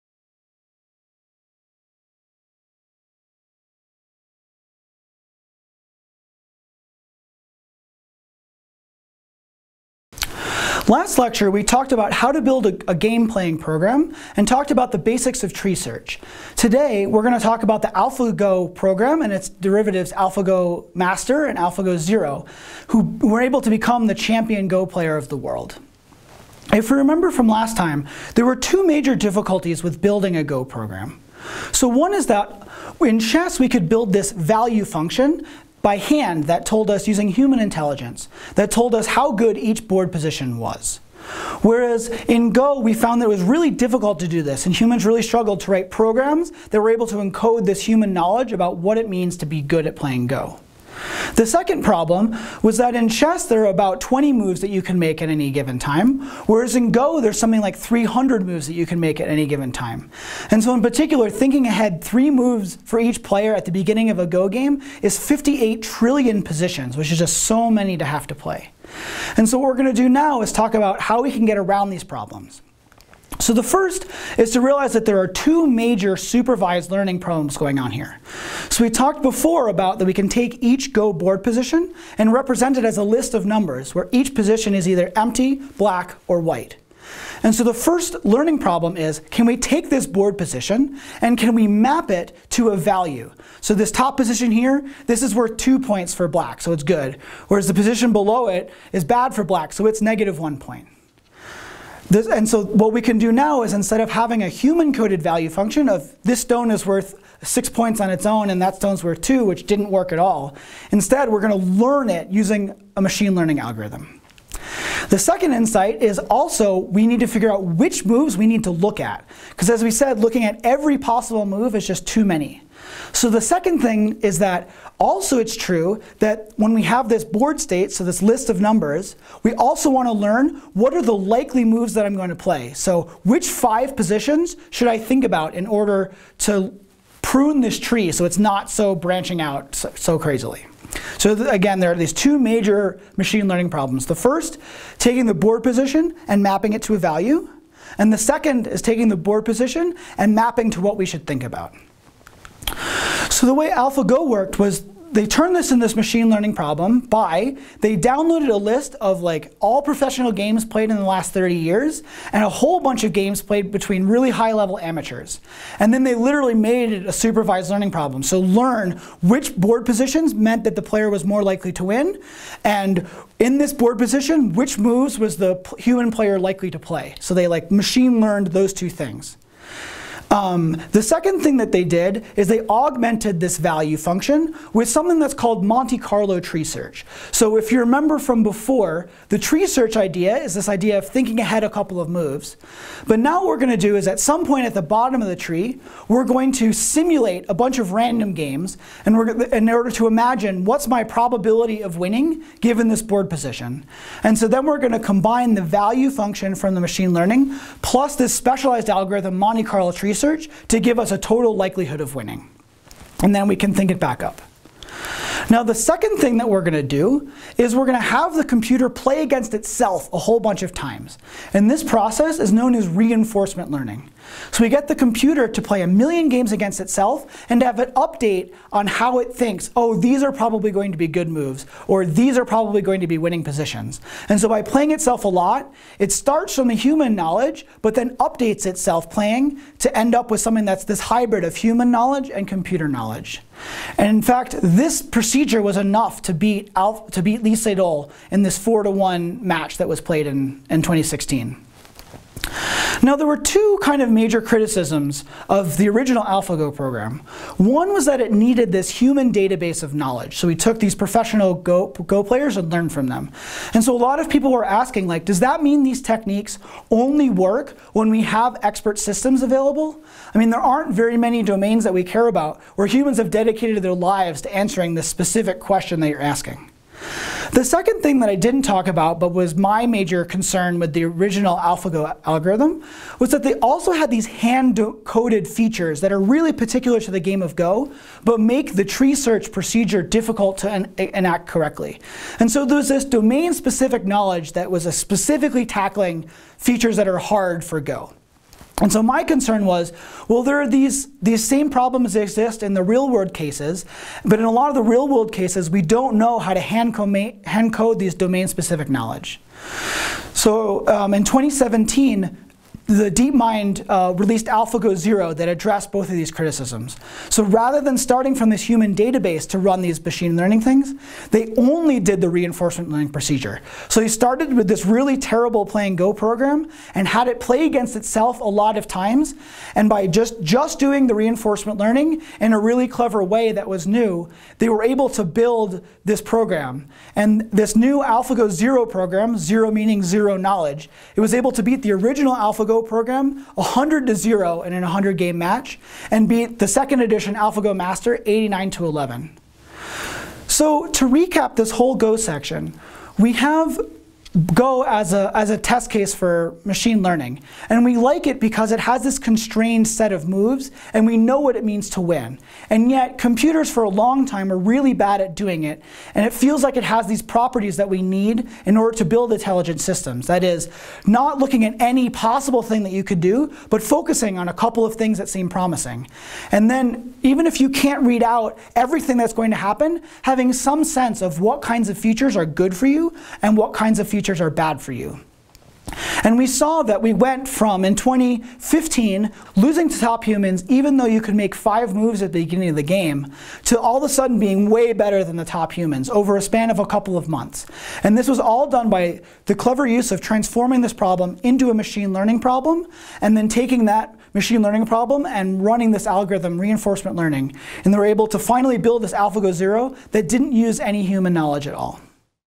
Fundamentals of Machine Learning: Building a Go Machine Learning Program
Machine Learning is a fast growing, rapidly advancing field that touches nearly everyone's lives. There has recently been an explosion of successful machine learning applications - in everything from voice recognition to text analysis to deeper insights for researchers. While common and frequently talked about, most people have only a vague concept of how machine learning actually works.

In this tutorial, Dr. Artemy Kolchinsky and Dr. Brendan Tracey outline exactly what it is that makes machine learning so special in an accessible way. The principles of training and generalization in machine learning are explained with ample metaphors and visual intuitions, an extended analysis of machine learning in games provides a thorough example, and a closer look at the deep neural nets that are the core of successful machine learning. Finally, it addresses when it's appropriate to use (and not use) machine learning in problem solving, as well as an example of scientific research incorporating machine learning principles.

For more complexity science videos, go to courses.complexityexplorer.org